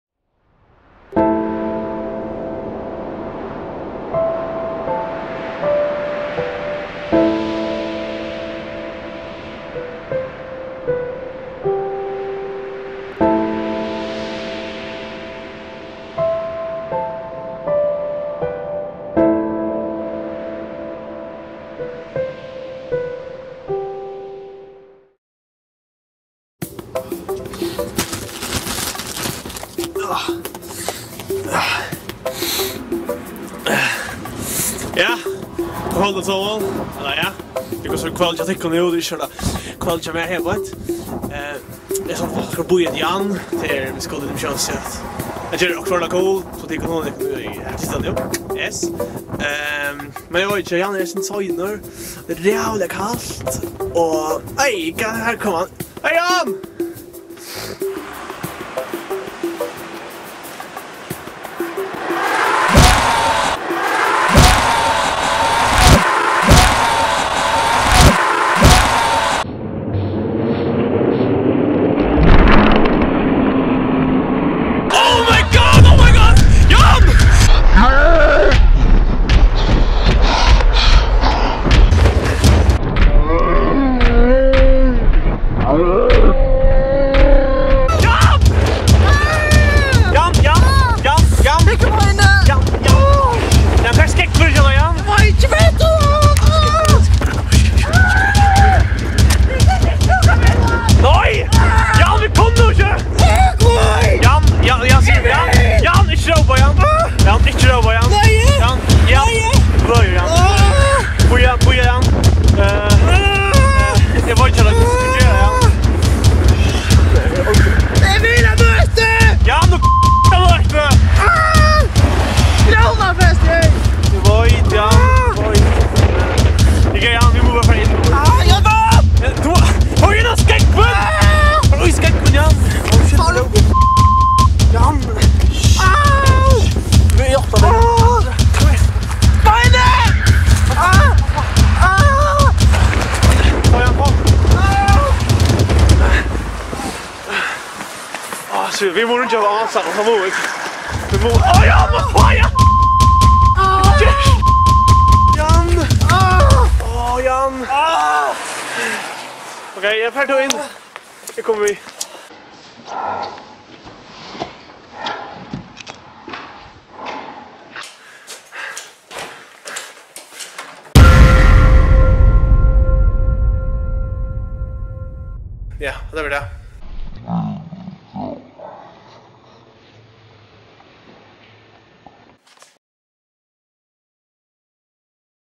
Jan & Arnholt!
This is a little video me and my friend Jan put together, to a Christmas-party at Glyvra Youthhouse.
- Enjoy! :D